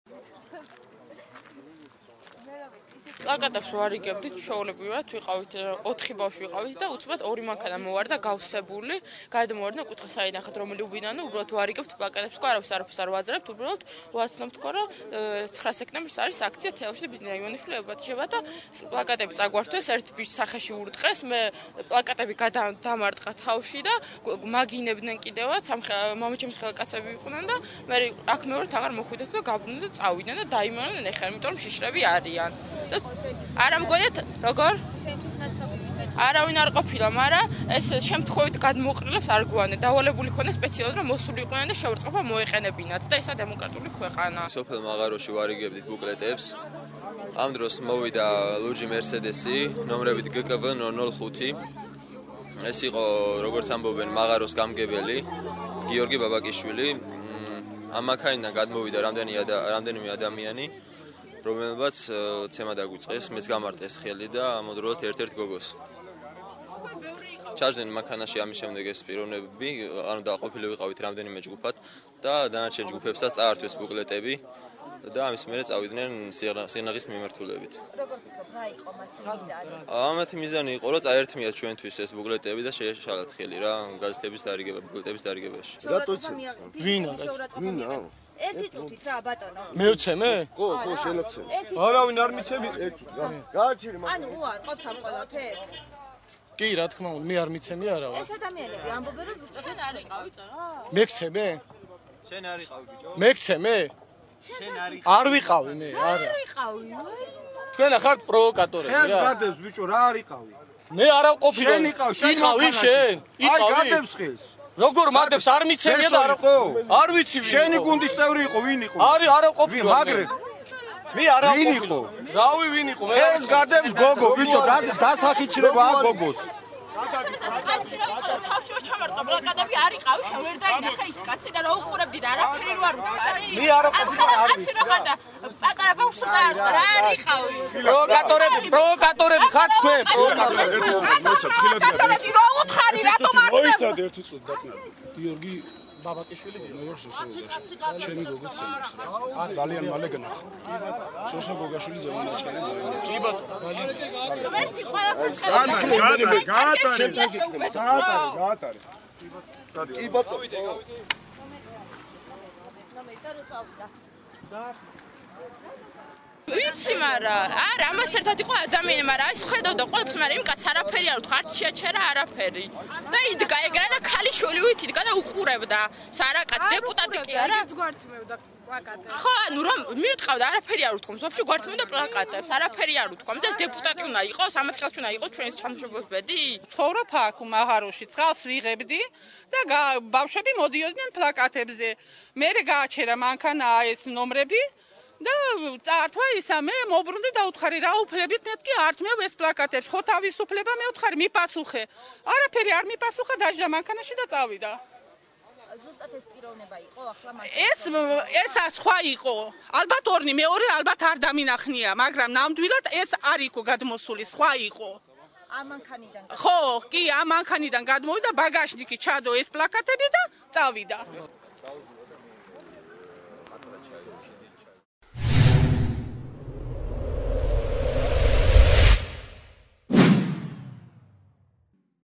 თქვენ პროვოკატორები ხართ
სიღნაღის რაიონის სოფელ ქვემო მაღაროში კოალიცია ,,ქართული ოცნების" ახალგაზრდა აქტივისტების  განცხადებით, მათ ფიზიკური და სიტყვიერი შეურაცხყოფა მიაყენა სოფლის დეპუტატის,  გიორგი ბაბაკიშვილის თანამგზავრმა მამაკაცმა.